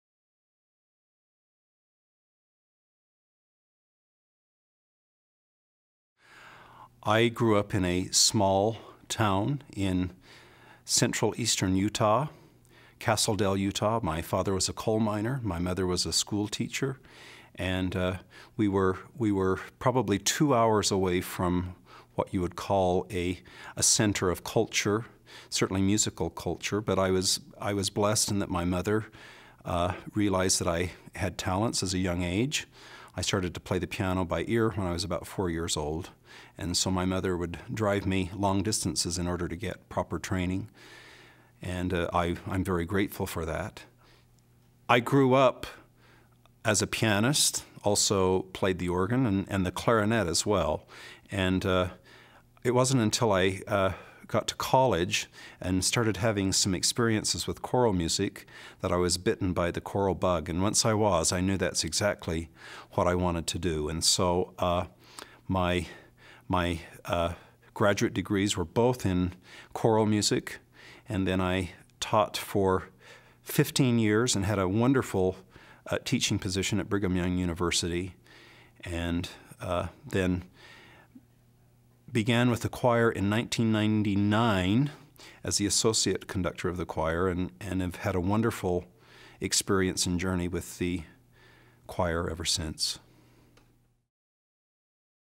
Mack Wilberg Biography: Getting into Music
Mack Wilberg, Music Director of the Mormon Tabernacle Choir, talks about his musical background and the path that led to his current position.